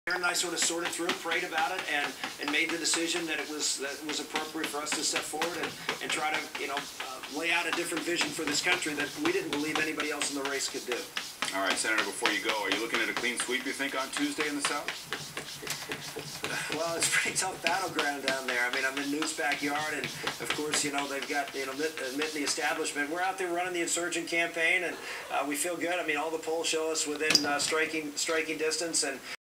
Petit Beurre disapproves of Rick Santorum
My cat Petit Beurre saw Rick Santorum on MSNBC and went berserk.